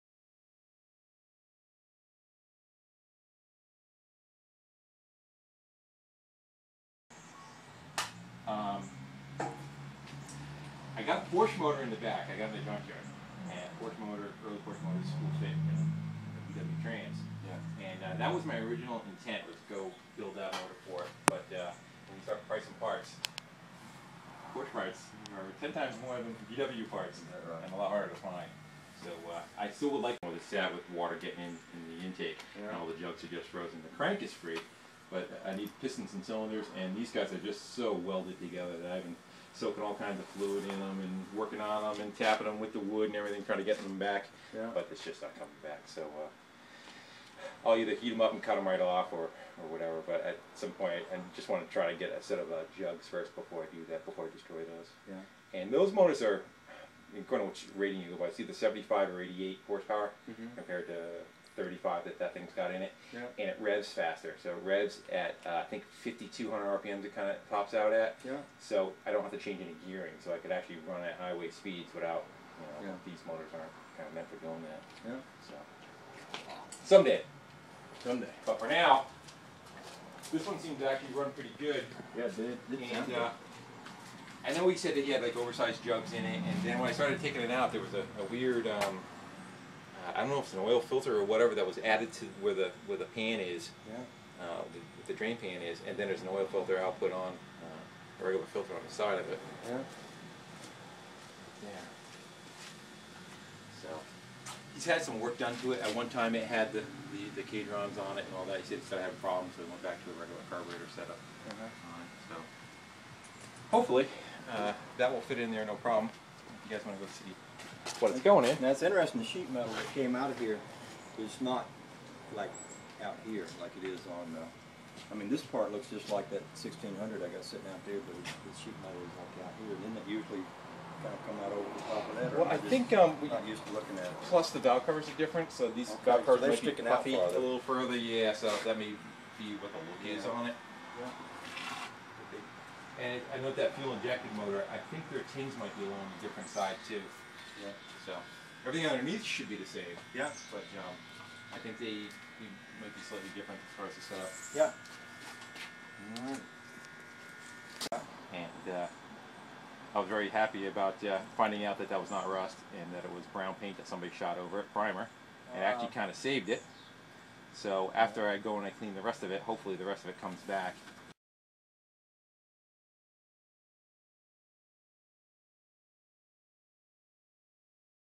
Visiting mustie1 (Darren) to get a VW with rpeek (Dad)
This was a good trip, met an amazing guy and picked up a VW while we were there